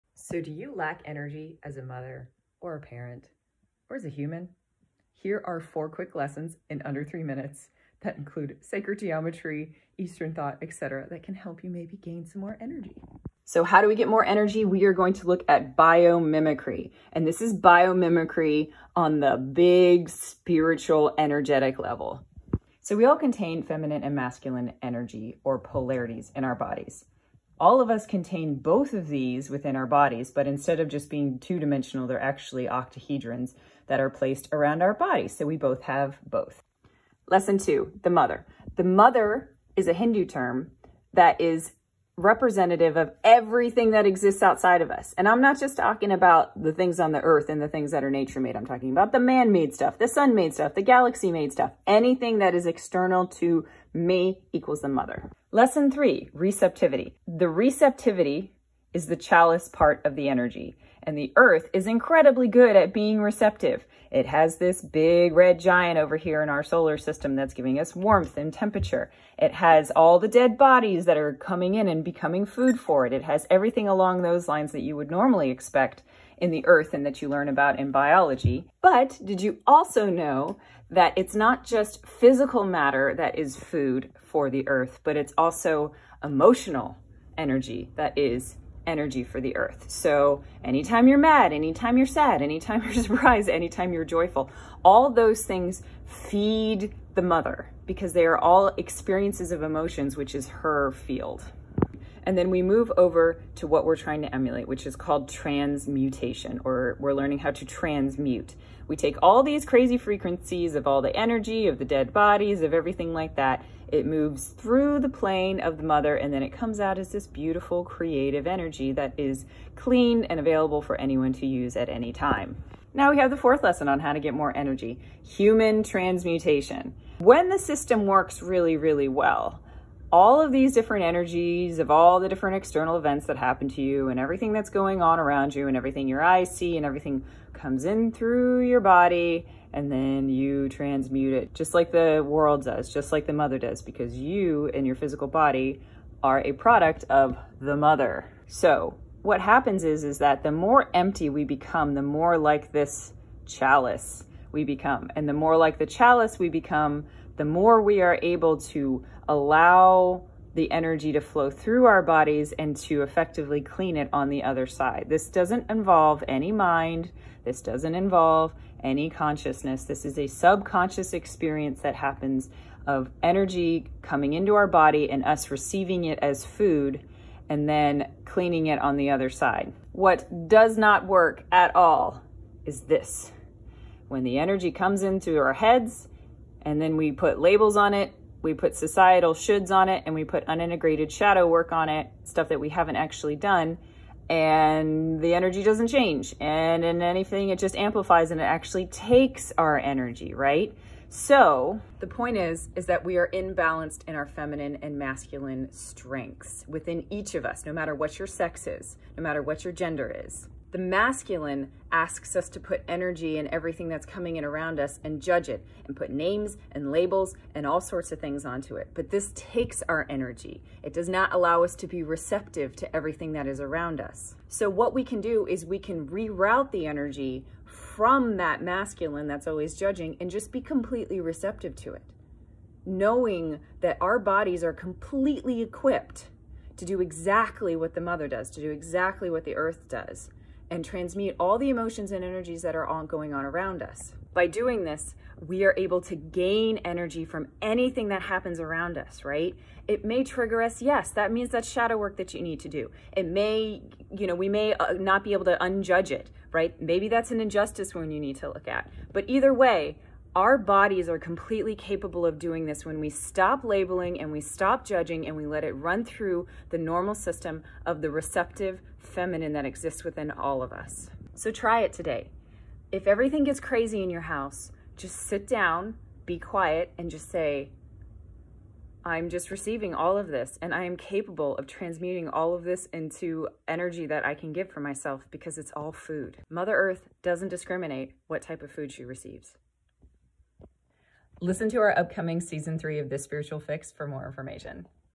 Get more energy!
How to use biomimicry and sacred geometry o get more energy